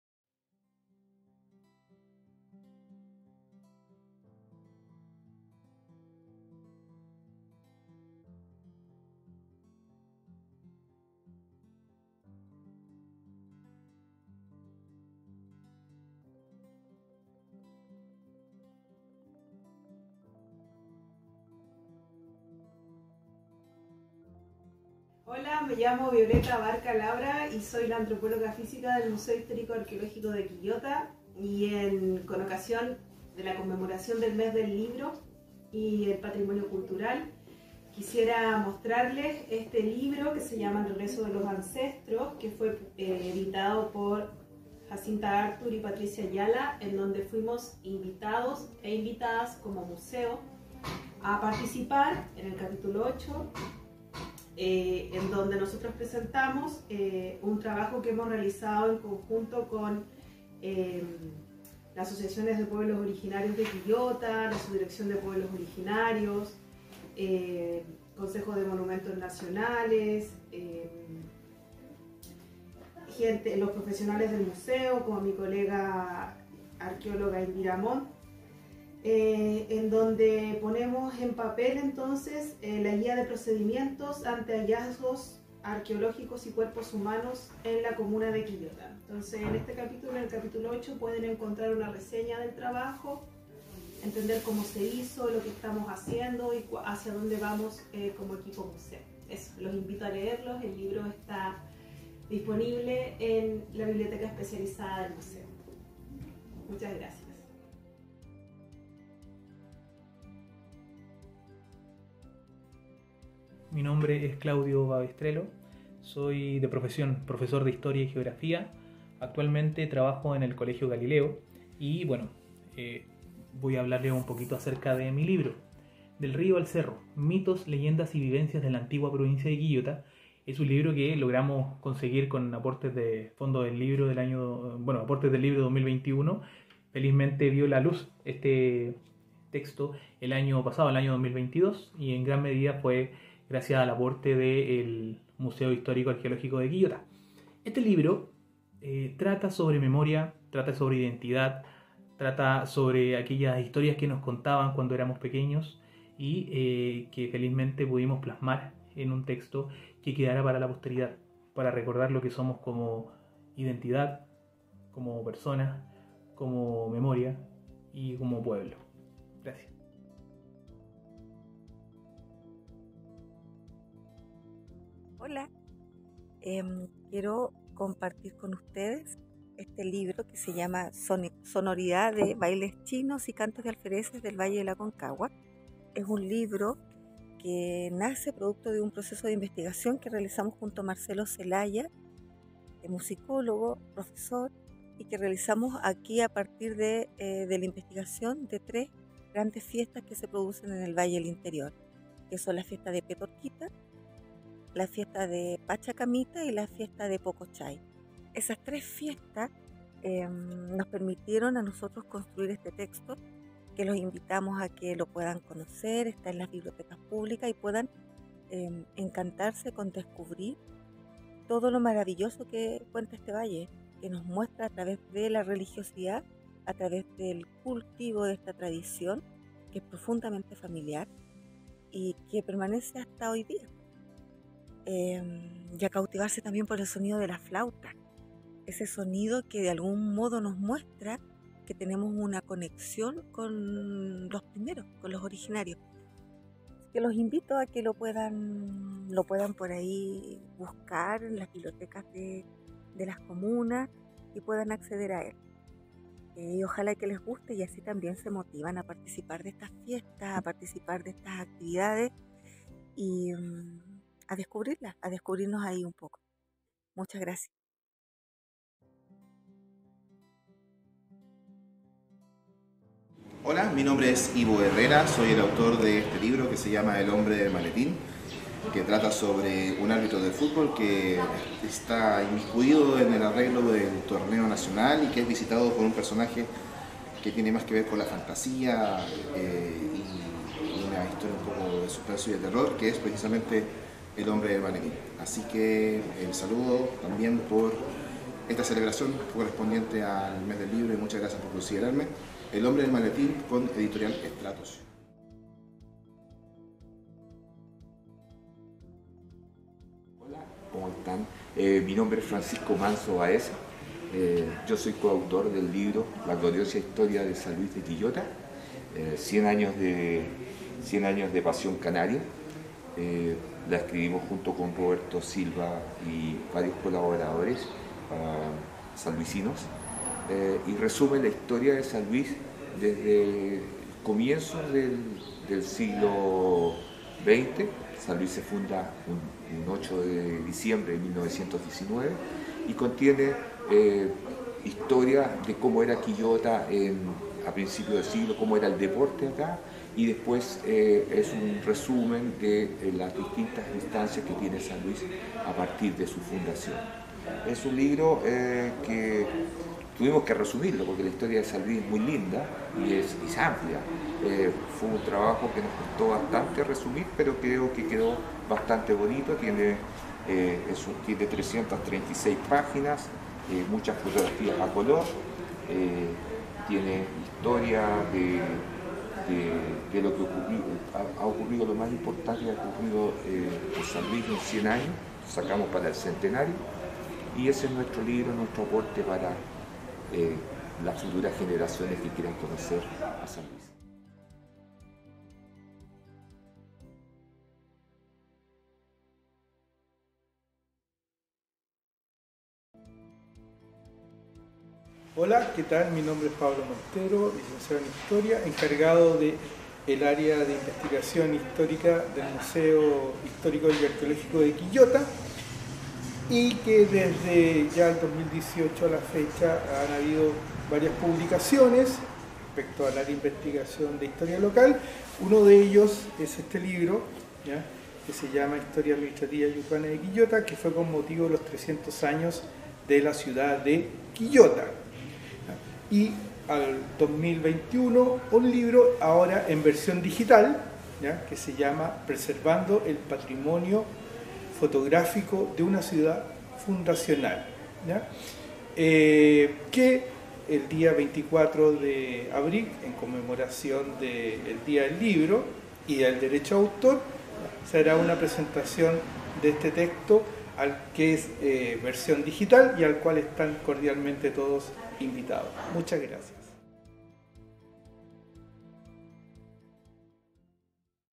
Día del Libro y Derecho de Autor
Testimonios de autores locales, de la muestra temporal en el Museo Histórico Arqueológico de Quillota.